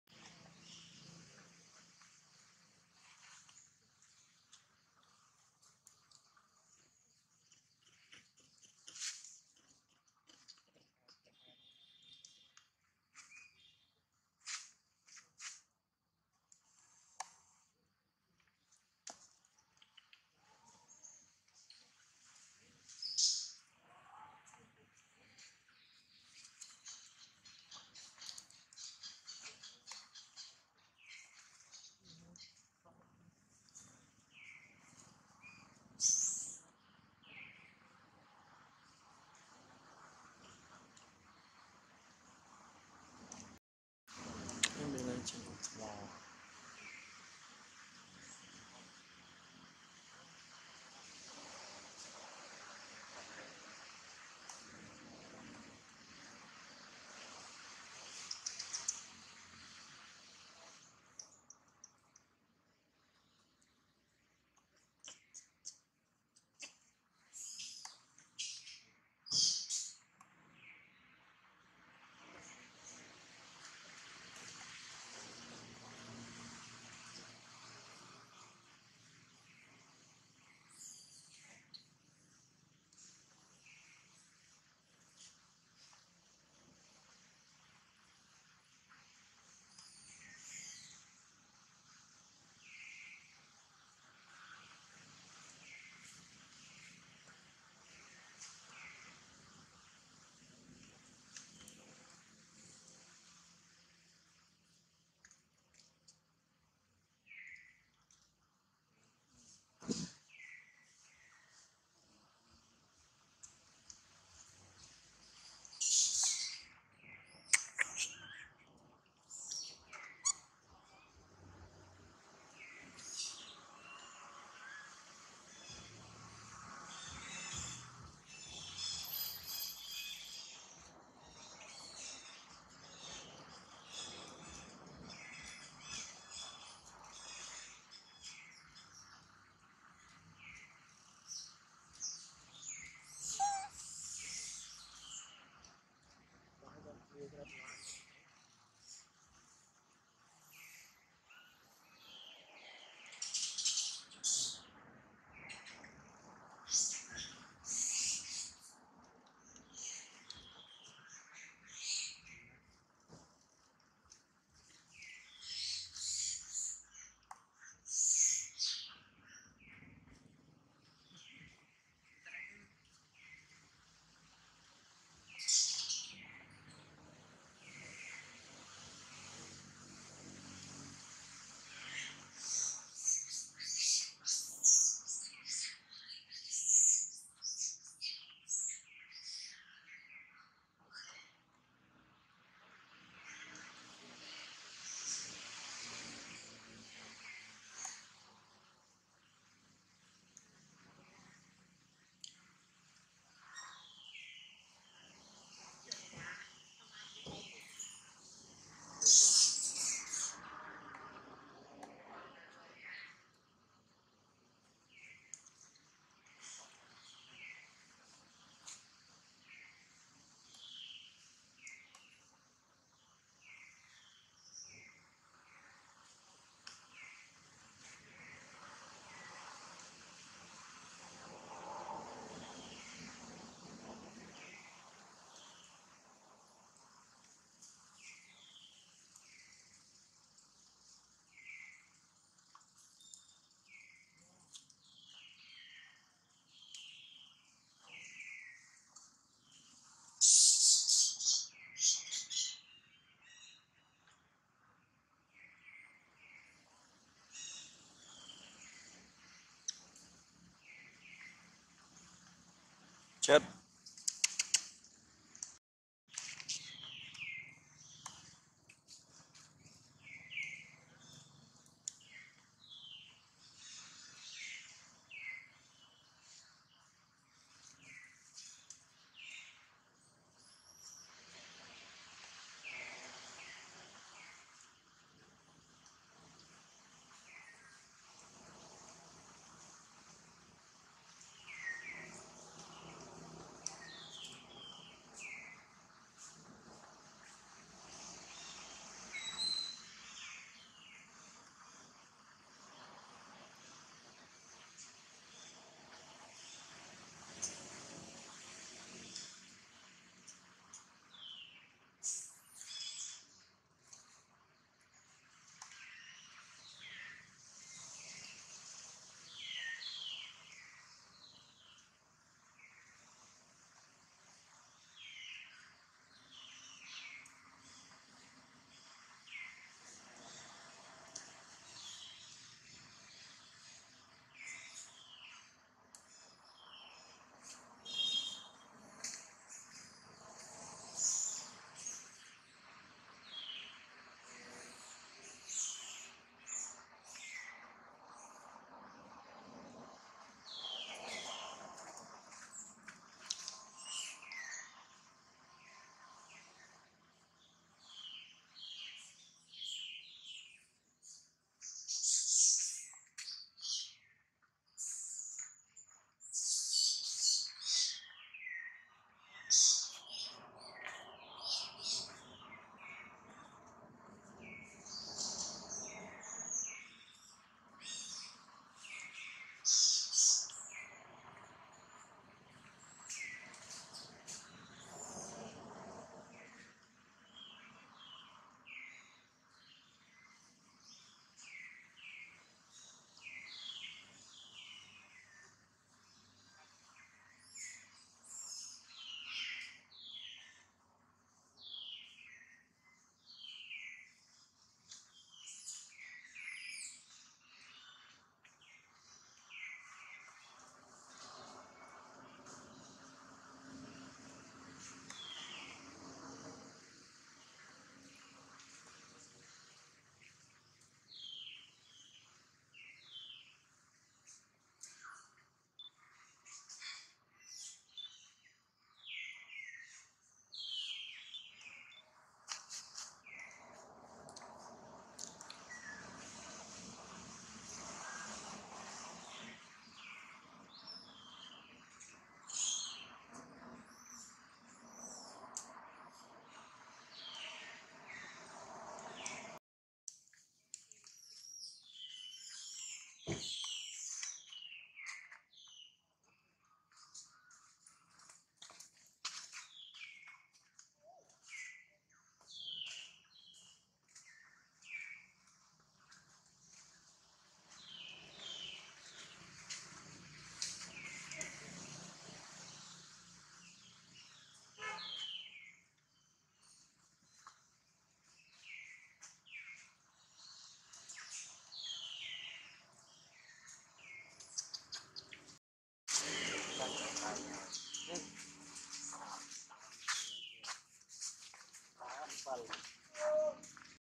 BIG MISTAKE...IT'S REALLY MUM JADE MISTAKE. POOR BABY JINX COLLAPSE FROM JADE ON THE GROUND AND CRY
Everyday Monkey